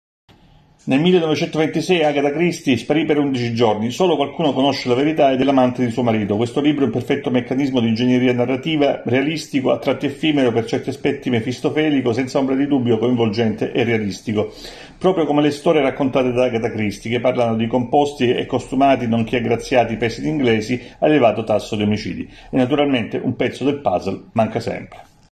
Il caso Agatha Christie di Nina de Gramont (Neri Pozza)
Nel 1926 Agatha Christie sparì per undici giorni. Solo qualcuno conosce la verità ed è l'amante di suo marito. Questo libro è un perfetto meccanismo di ingegneria  narrativa, realistico, a tratti effimero, per certi aspetti mefistofelico, senza ombra di dubbio coinvolgente e realistico proprio come le storie raccontate da Agatha Christie che parlano di composti e costumati nonché aggraziati paesini inglesi a elevato tasso di omicidi. E naturalmente un pezzo del puzzle manca sempre …